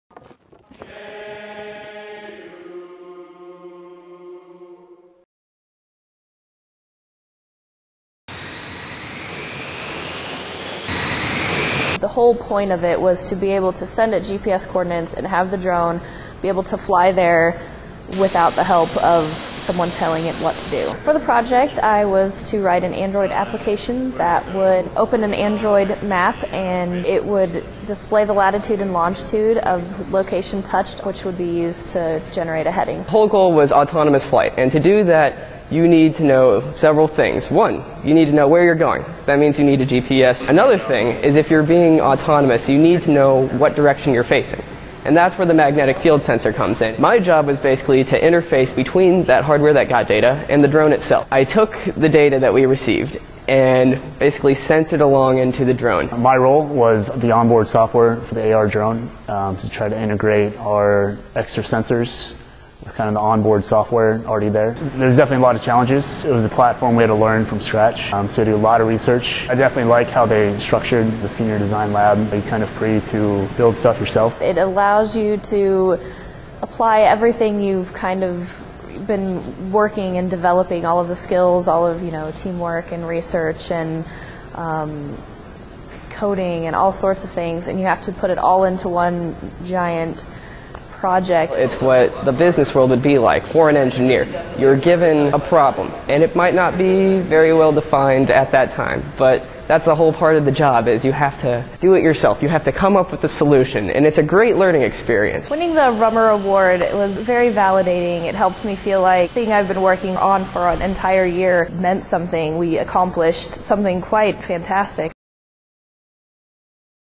University of Kansas seniors build autonomous drone
Successfully turning a remote-controlled toy helicopter into an autonomous drone earned three computer engineering (CoE) students departmental honors and a $1,000 cash award. To learn more about their project, go